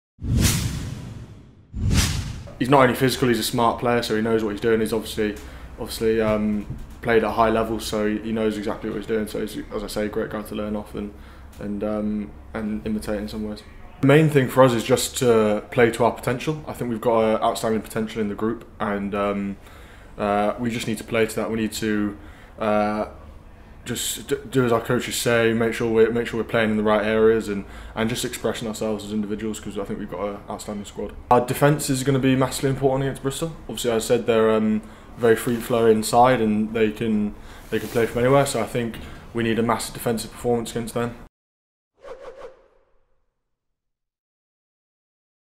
Ted Hill - We need to play to our potential
Ted Hill looks forward to returning to Gallagher Premiership action after a two week break.